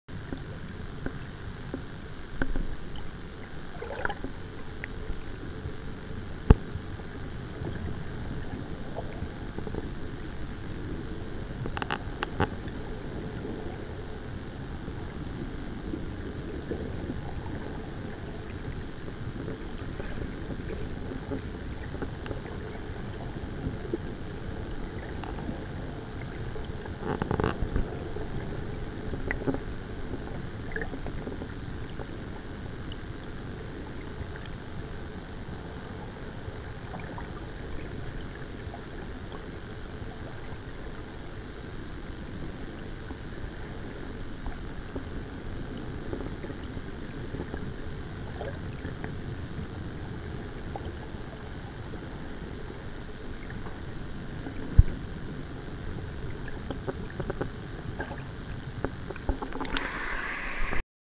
Alewives and friends in Camden Harbor
Alewives and friends in Camden Harbor trying to get to Megunticook Lake.